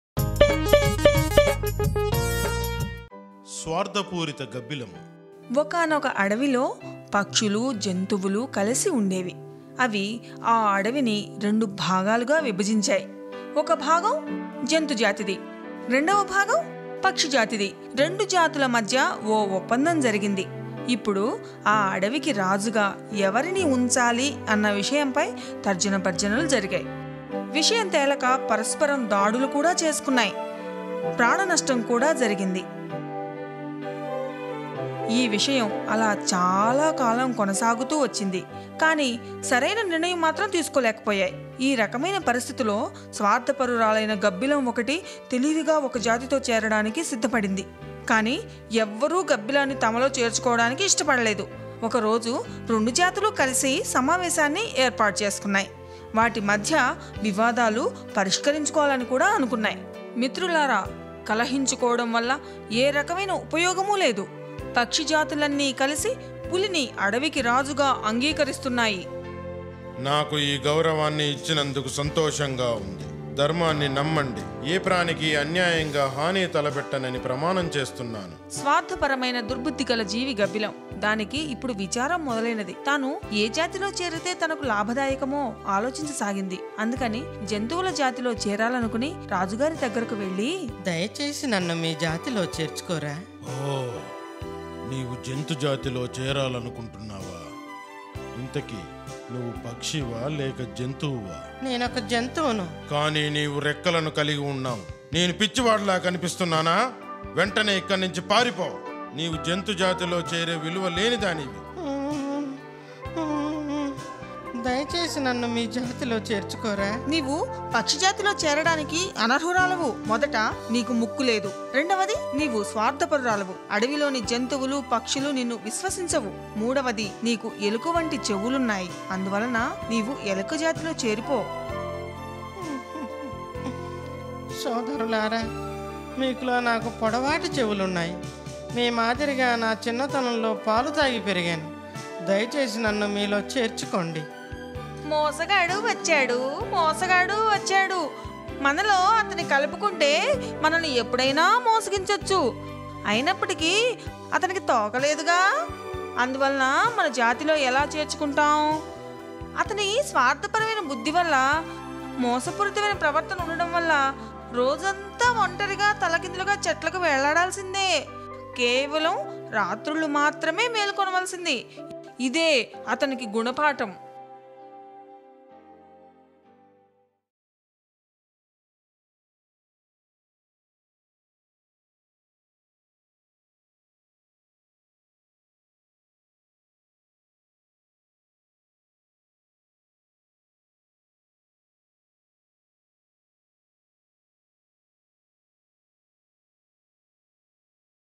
Moral Stories For Children | Swardhapuritha Gabbilamu | Animated Telugu Stories | Balamitra
Moral Stories For Children featuring Swardhapuritha Gabbilamu Story forms best Animated Telugu Stories For Kids available only on Balamitra.